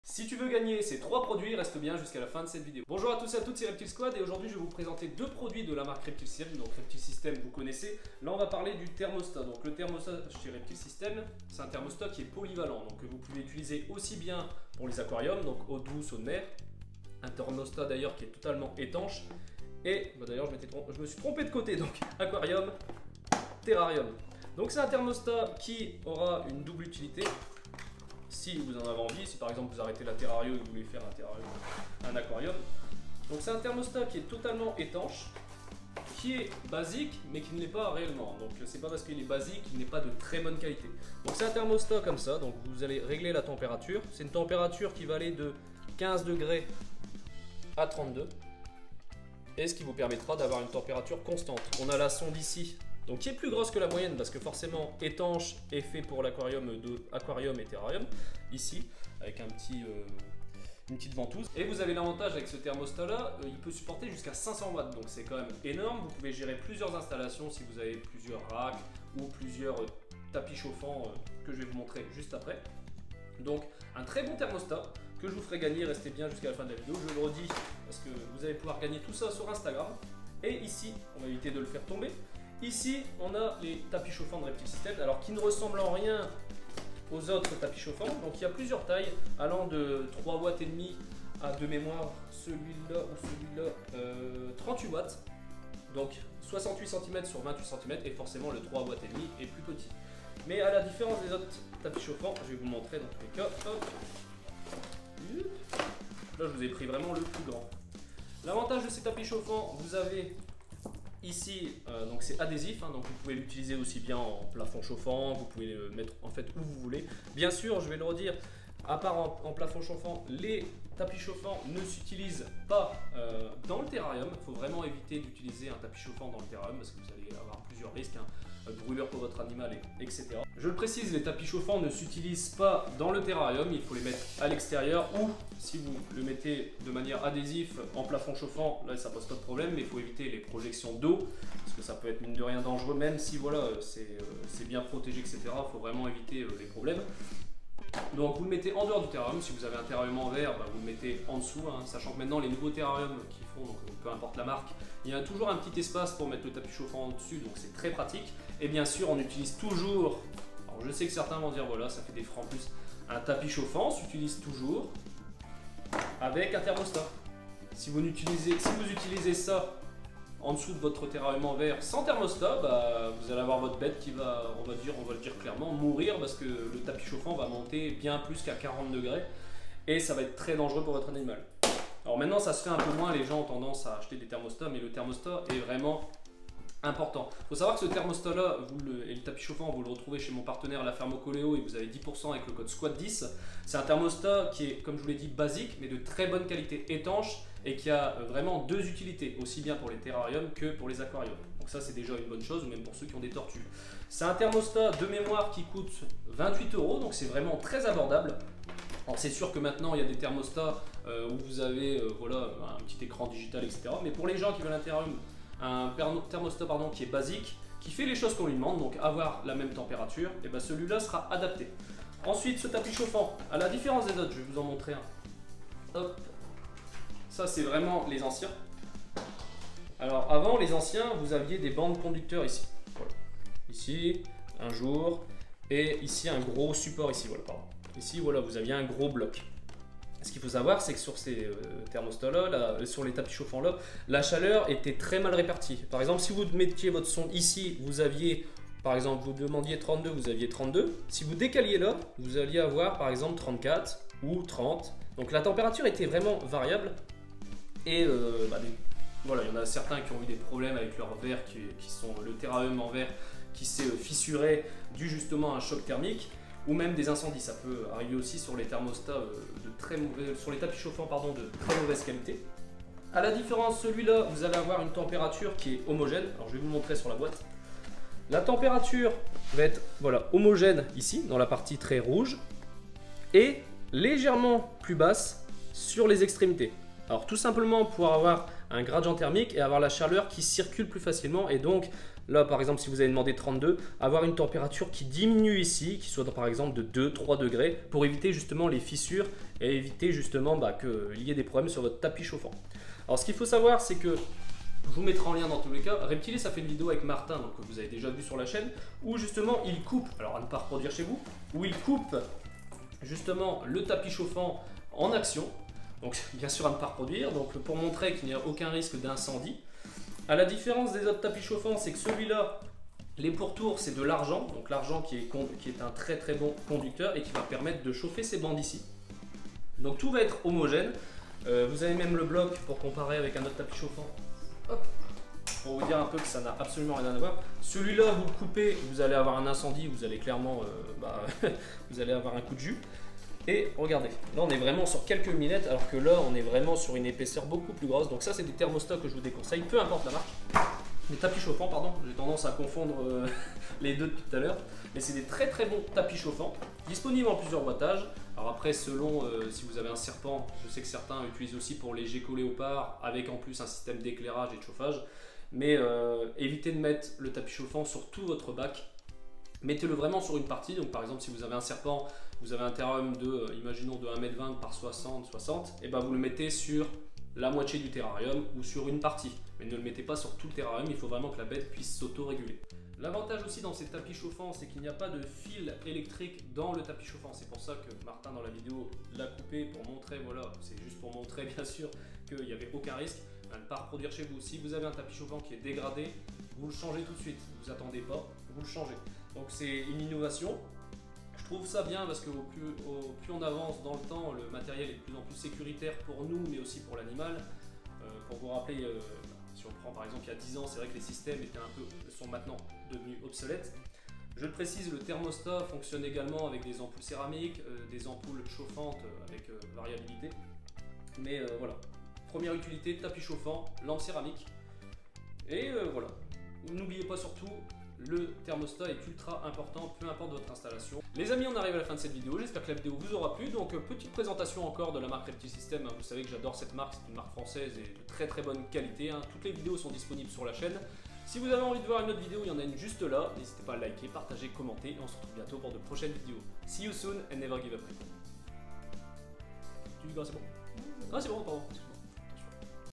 THERMOSTAT ET TAPIS CHAUFFANT ÉCONOMIQUE??(Je te les offre) - REPTILE SQUAD
Dans cette vidéo je te présente le tapis chauffant et le thermostat de Reptile Systèms, l'un des meilleurs rapport qualité prix sur le marché.
Tu peux les gagner alors va vite sur le compte Instagram surveiller ça.

MES PARTENAIRES:

-10% avec le code ''SQUAD10'' (à partir de 29 euros d'achat)

-10% avec le code SQUAD10

-10% avec le code SQUAD10